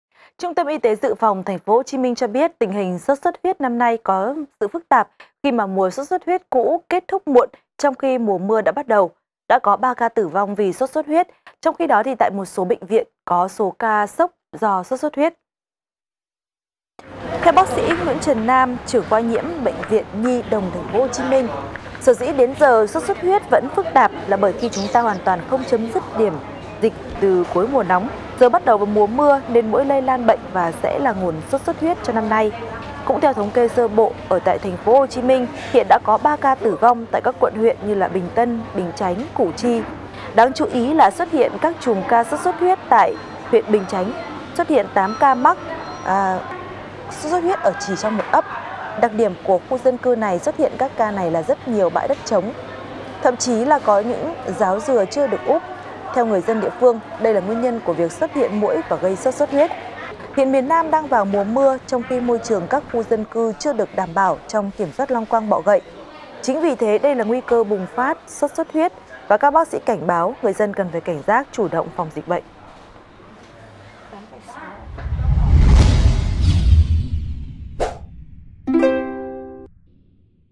TP HCM: Mùa sốt xuất huyết kết thúc muộn, nhiều ca biến chứng nặng| VTC14
VTC14 | TP HCM: MÙA SỐT XUẤT HUYẾT KẾT THÚC MUỘN, NHIỀU CA BIẾN CHỨNG NẶNG